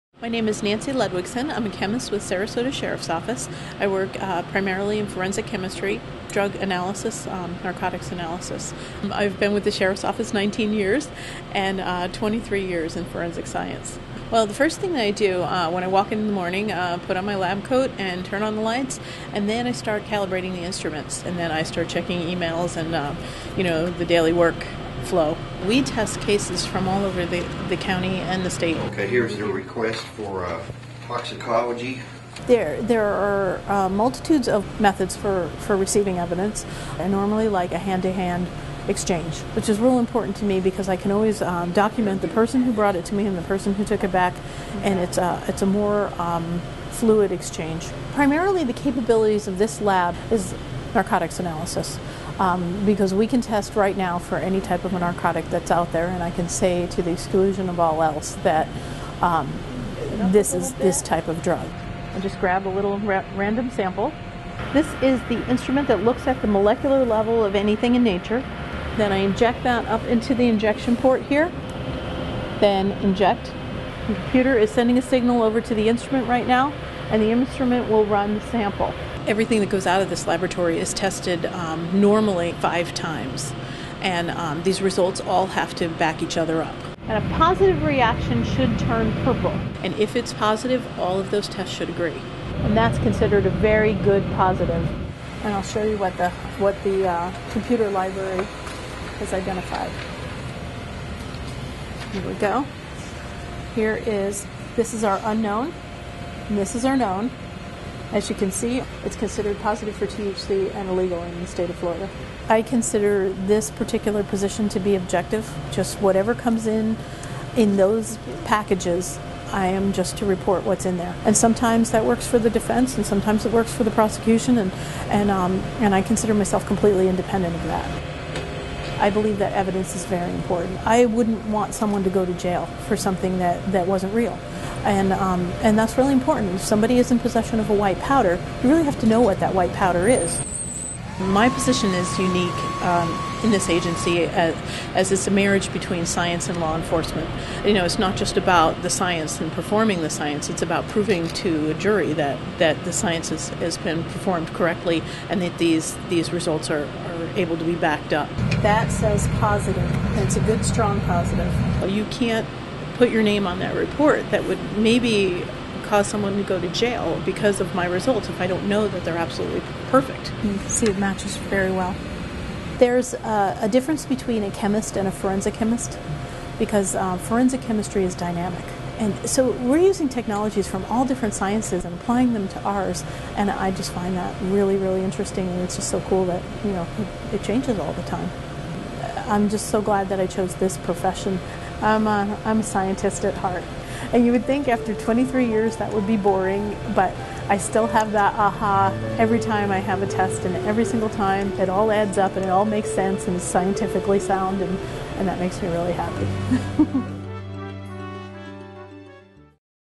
Behind the Scenes - Forensic Chemistry Lab
In our continuing series that takes you behind the scenes at the Sarasota County Sheriff's Office, this videotakes you through "a day in the life" of Forensic Chemist Nancy Ludwigsen.